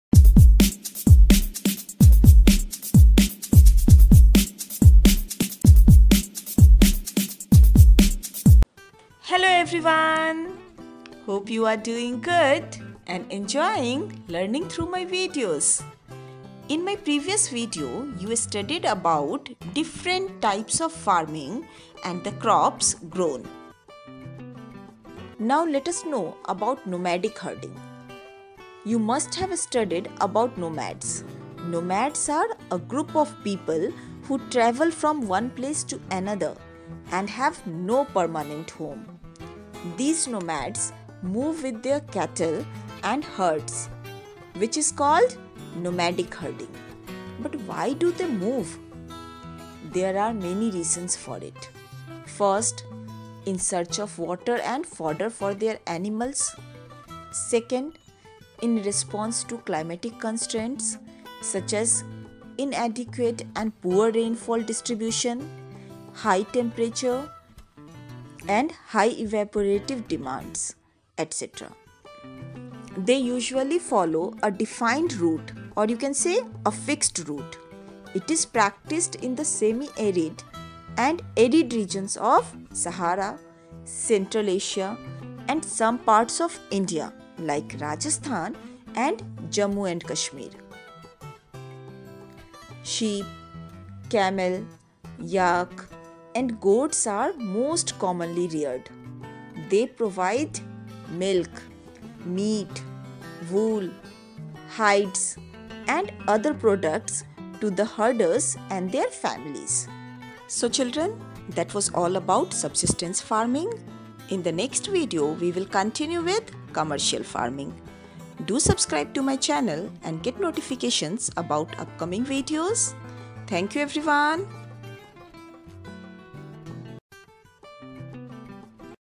What is Nomadic Herding? | Types of farming | Agriculture | Geography
Dear learners,
This video is about Nomadic Herding. You will learn about the life of Nomads and the problems faced by them.

Click on the following links to check more videos of classes 6 to 10.